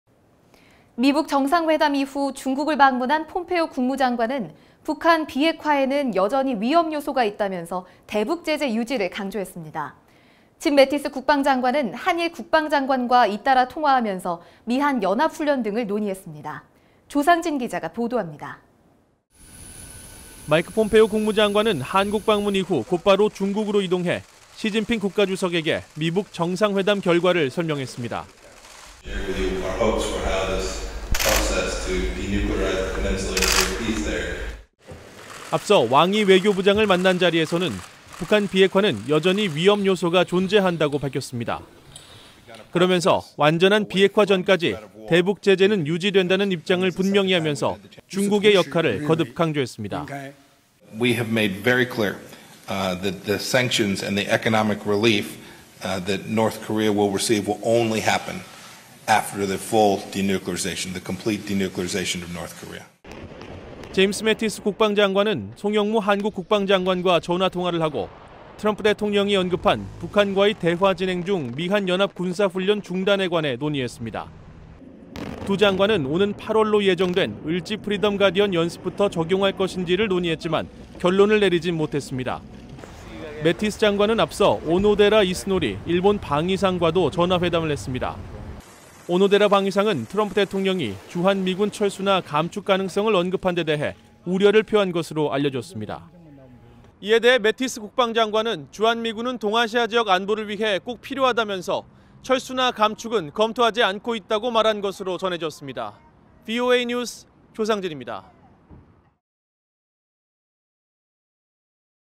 [VOA 뉴스] “북한 비핵화 위험요소 여전”
중국을 방문한 마이크 폼페오 미 국무장관은 왕이 중국 외교부장을 만나 북한의 비핵화에 있어 여전히 위험 요소가 존재한다고 밝혔습니다. 또 북한 문제에 여전히 할 일이 많다면서 중국의 역할을 강조하고, 북한 비핵화가 완결될 때까지 대북제재는 유지돼야 한다는데 공감했다고 말했습니다. 짐 매티스 미 국방부장관은 송영무 한국 국방부 장관, 오노데라 이쓰노리 일본 방위상과 전화 회담에서 미한 연합군사훈련 지속 여부를 한국과 조율하고 있으며 주한미군 철수, 감축은 전혀 고려하지 않고 있다고 밝혔습니다. (취재 : 조상진 / 영상취재 : 이상훈 / 영상편집 : 김정호)